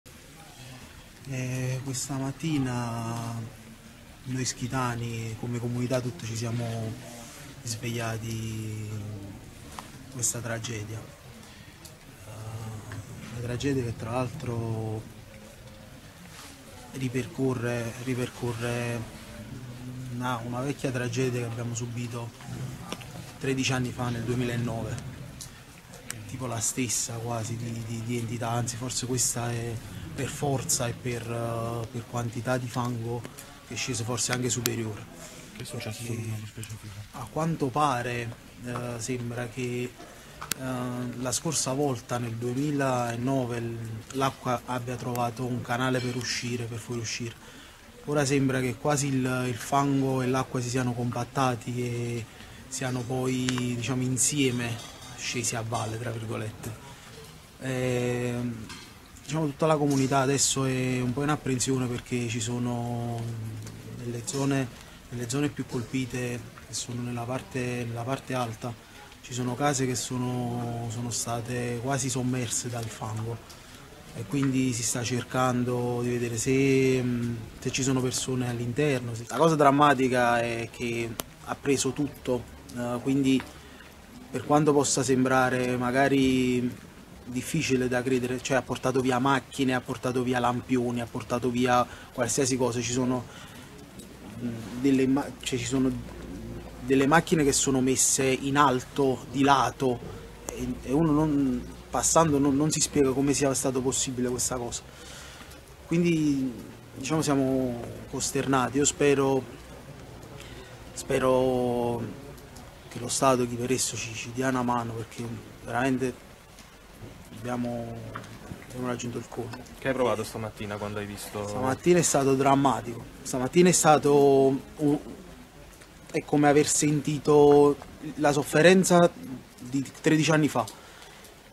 «È stato drammatico, case quasi sommerse dal fango», la testimonianza di un abitante di Ischia
«È stato drammatico, case quasi sommerse dal fango», la testimonianza di un abitante di Ischia dopo la frana